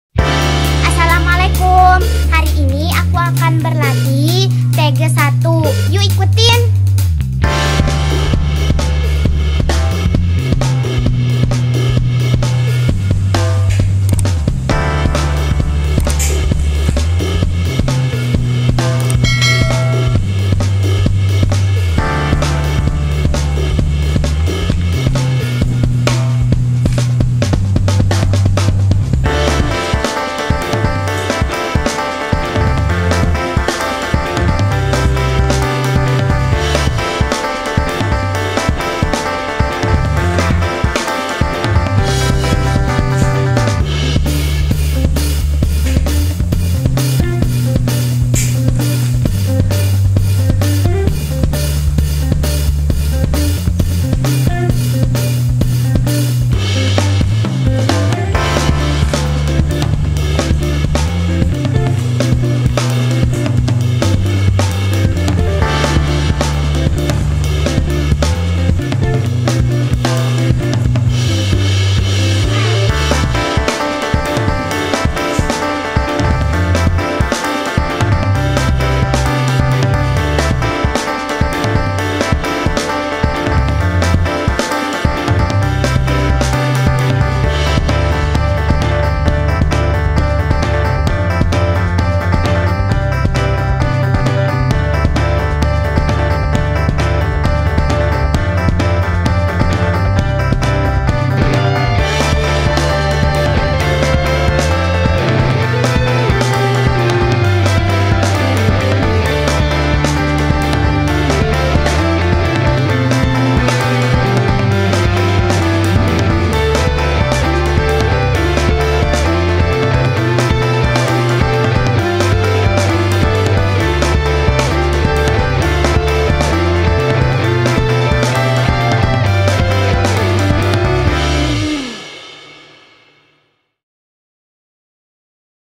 Latihan Taekwondo Taegeuk 1 | Olahraga Beladiri Korea Taegeuk Poomsae
Taekwondo merupakan olahraga beladiri yang berasal dari Korea. Olahraga ini baik untuk perkembangan motorik kasar anak, serta dapat meningkatkan konsentrasi.
Taekwondo merupakan olahraga beladiri dari Korea.